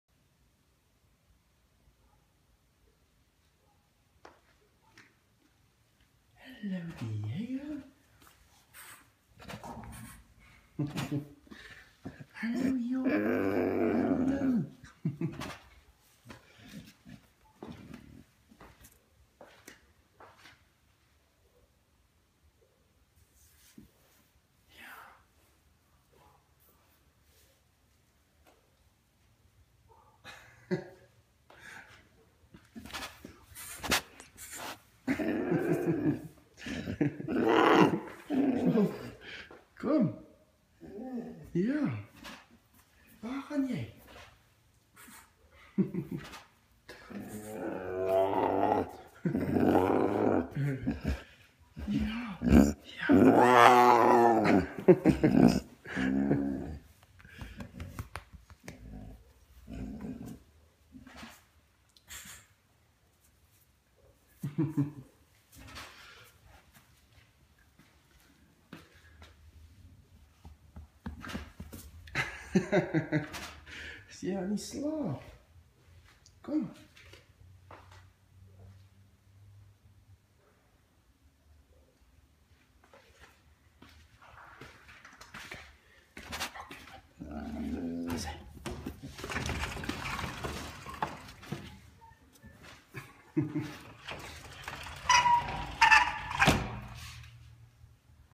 On my way downstairs I found a tiger on the step!
I was going downstairs ,Diego was taking a nap on one of the steps , He is as friendly as can be but covering the whole step